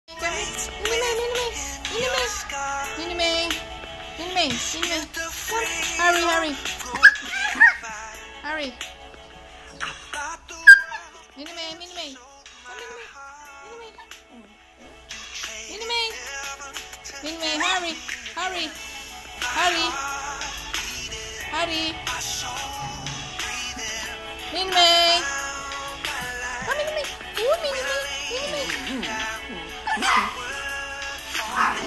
Barak getting mad while I call on other Pups!
Barak bites when he is jealous